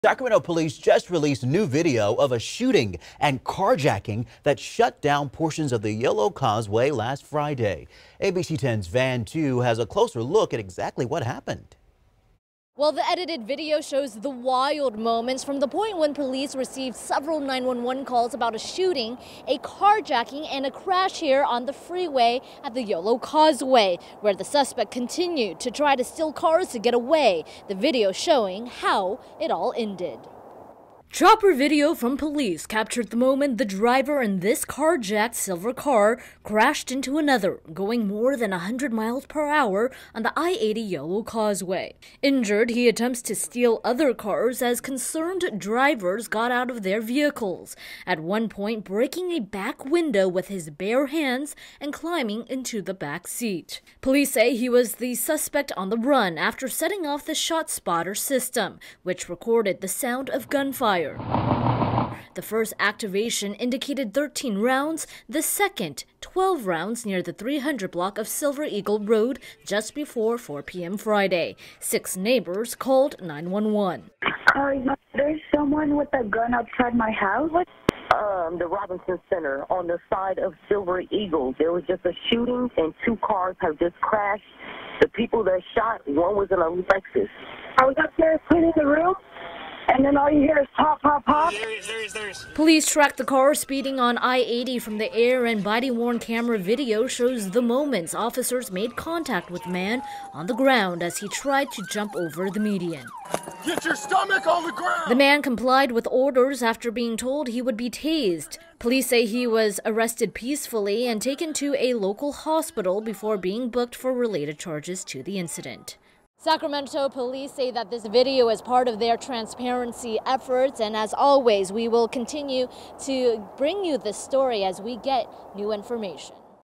Sacramento police release video showing lead up to suspected carjacker arrest along Yolo Causeway
The Sacramento Police Department has released an edited recap video showing the arrest of a suspected carjacker who apparently took off from the scene of a shooting in Sacramento and wound up crashing on I-80 near the Yolo Causeway.